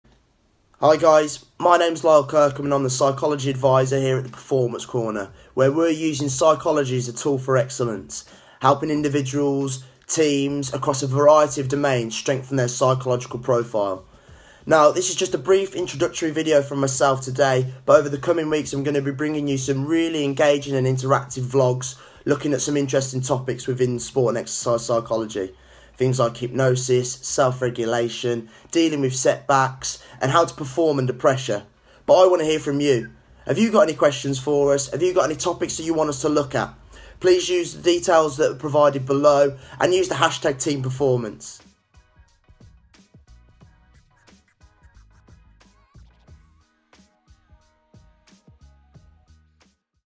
Lyle Kirkham - Performance Psychologist
Get in touch now if you are looking for support with your mental game. Lyle has a vast amount of knowledge and specialises in transferring that knowledge into activities and actions that athletes can use.